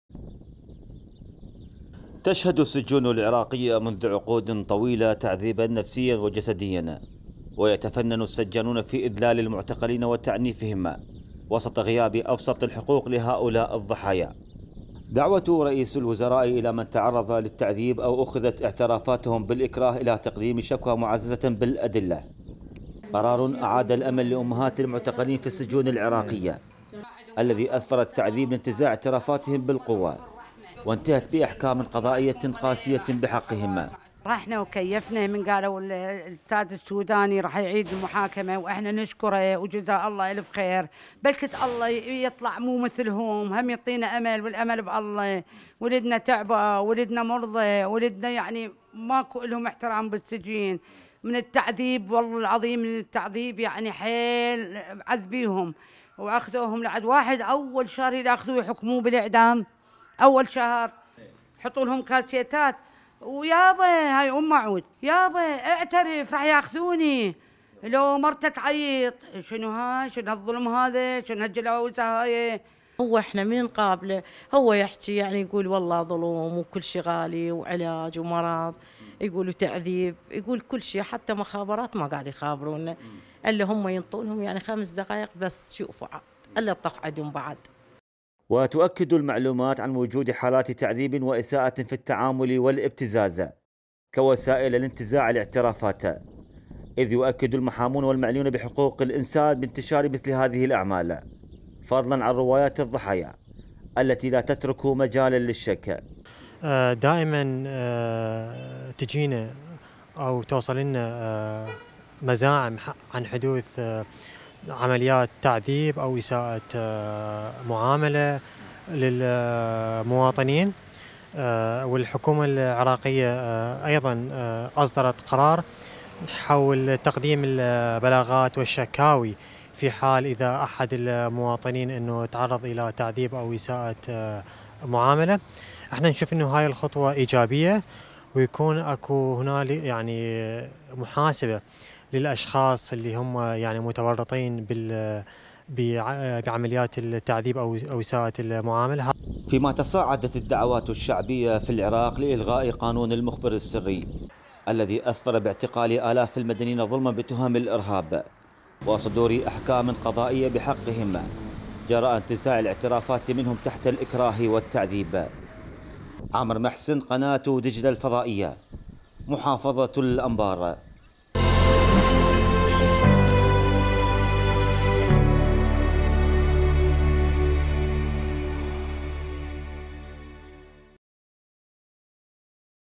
أهالي معتقلي الانبار يشيدون بقرار السوداني لفتح باب الشكاوى على إذلال ابنائهم
11258📺 افقي